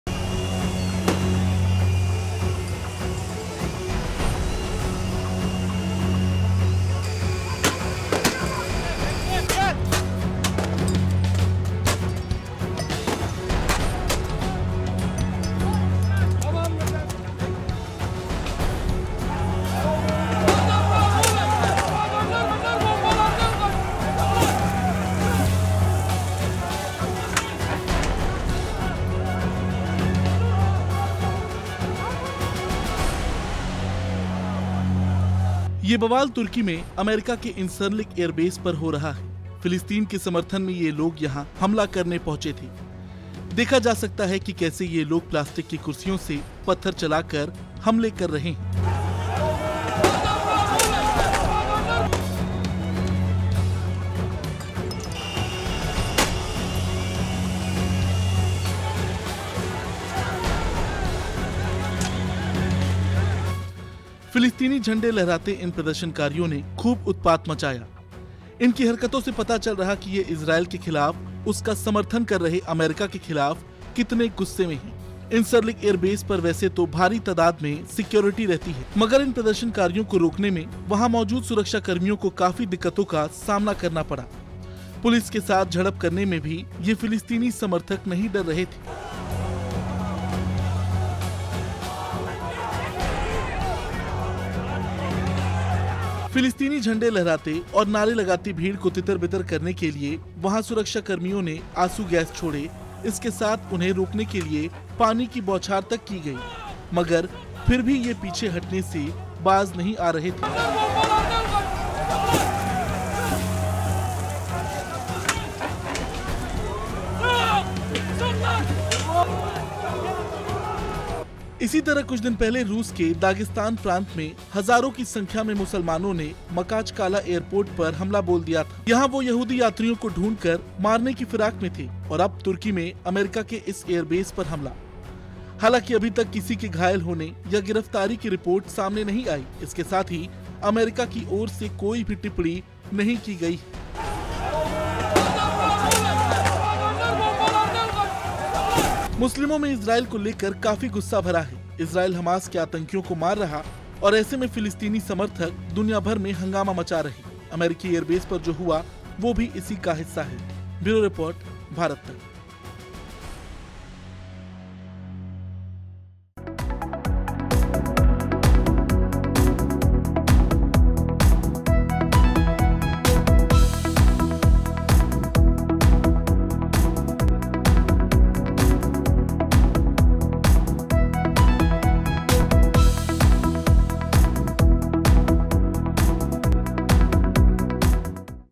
Turkey Attack On America: Turkey में मुसलमानों ने American Airbase की कर दी ऐसी तैसी, मचा गदर !
Turkey में मुसलमानों ने American Airbase की कर दी ऐसी तैसी, मचा गदर !